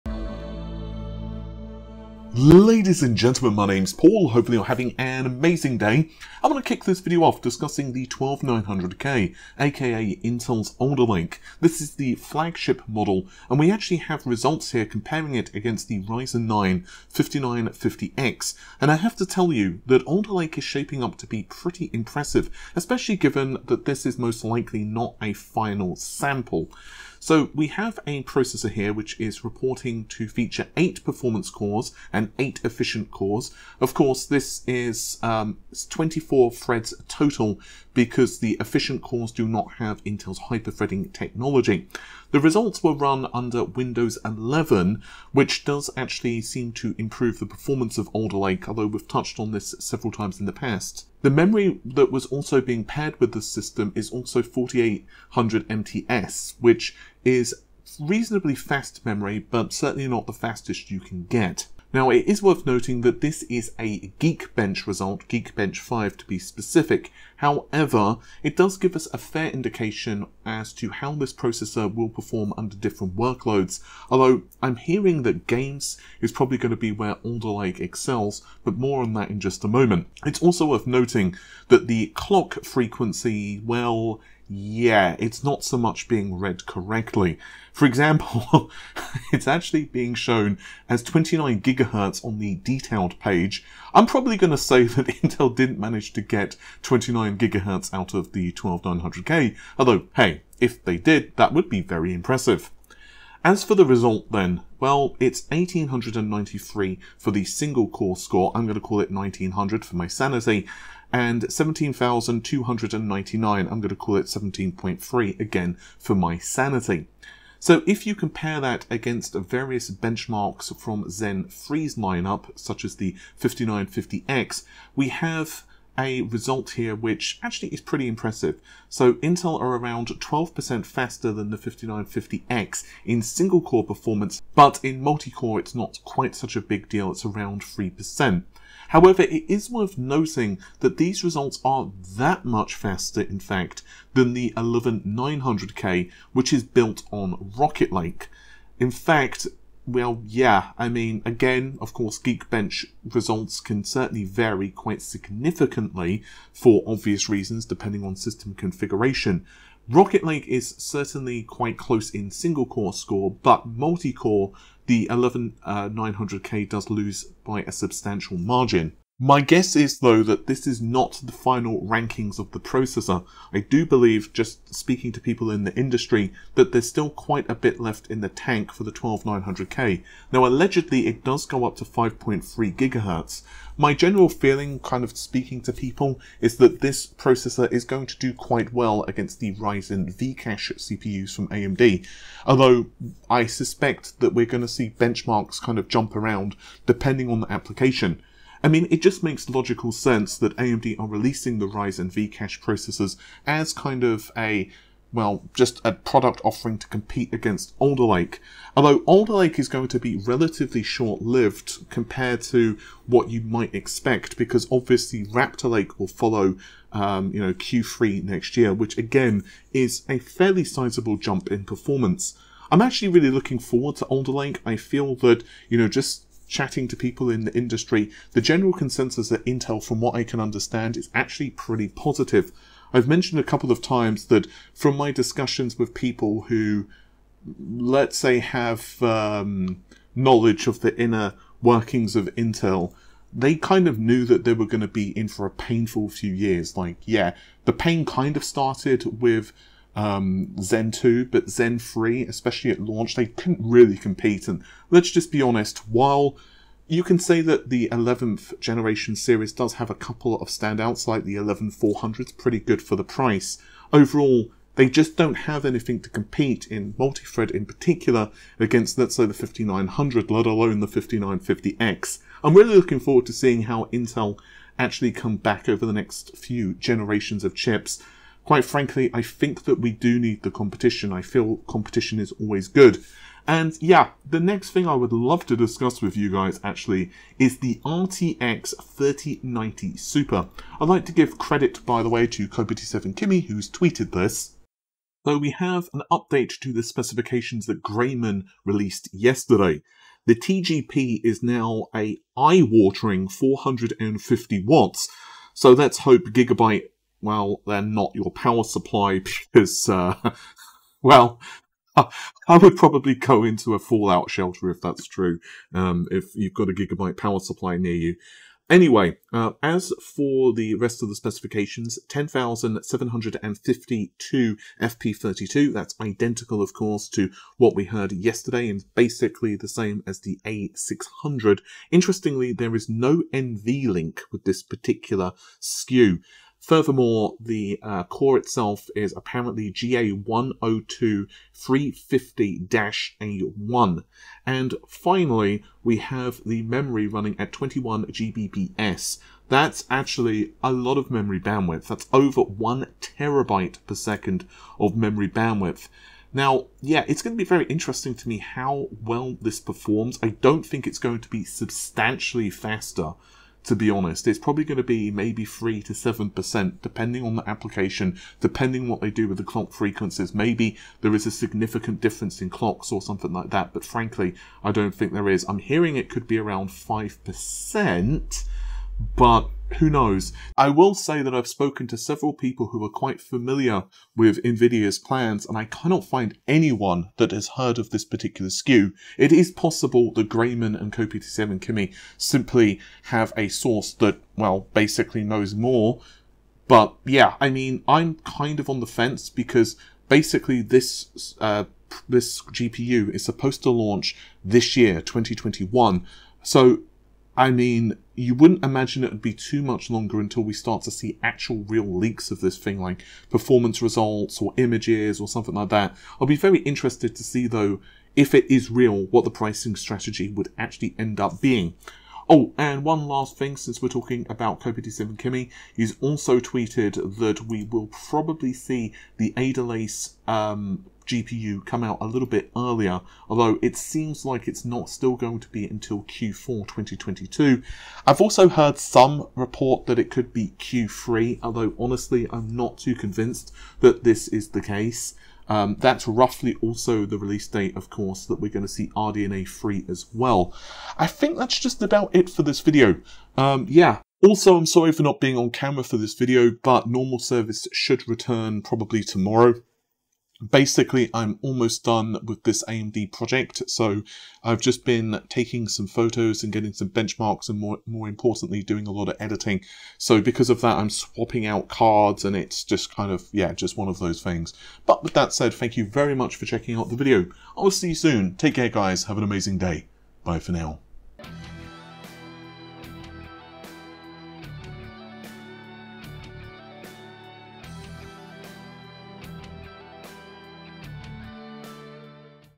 Intel Alder Lake WRECKS Zen 3 In Benchmarks | RTX 3090 Super RIDICULOUS Specs Leak
New benchmarks have surfaced for Intel Alder Lake's 12900K, and these performance results show it WRECKS the Zen 3 Ryzen 5950X processor from AMD . Intel has struggled to compete against Zen 3 Ryzen processors, so the 12900K Alder Lake CPU showing such strong performance against Ryzen Zen 3 processors is very promising. We finally know more about the specifications and how the hybrid tech works for Alder Lake, but can the 12900K and other Intel SKUs actually compete against Zen 3 and Zen 4 CPUs?

We also have a LEAK for the RTX 3090 SUPER, with more info on the specs for the rumoured RTX 30 refresh. The RTX 3090 is already a MONSTER in performance (and price), so what hardware specifications and improvements could we see for the RTX 3090 SUPER?

0:00 Start
0:06 Intel 12900K
4:44 RTX 3090 SUPER
7:05 RTX Lovelace